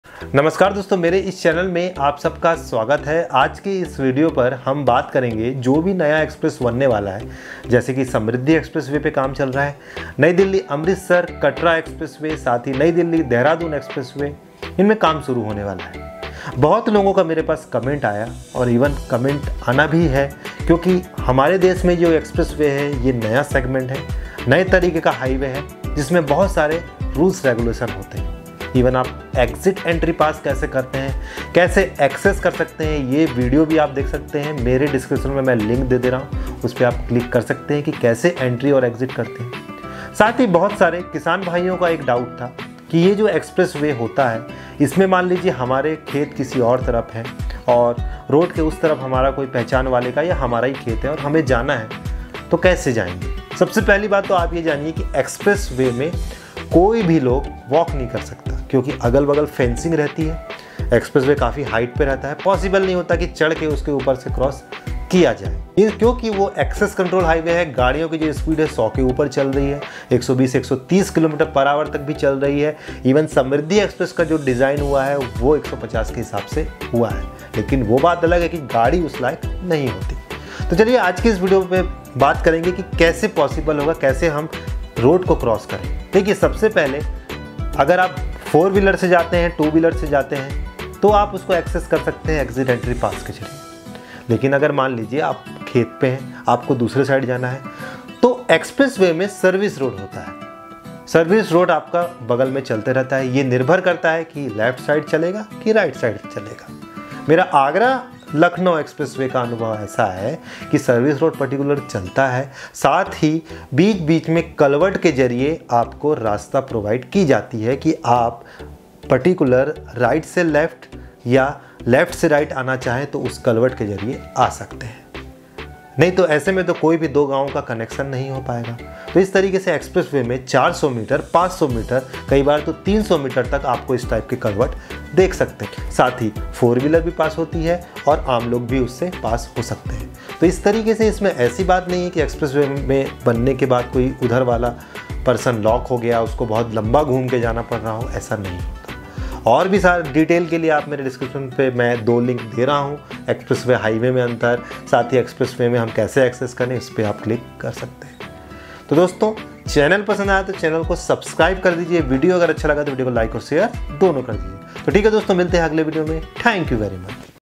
Express Way ! Know About Crossing Rule for People in Express Way
hi everyone in this video I am presenting expressway crossing rule by people farmer or any other because it is access control highway due to this reason people can't cross hence in this video you will know how to cross and connectivity between different villages all the three i.e Samruddhi expressway Amritsar expressway and  Dehradun expressway construction is going on.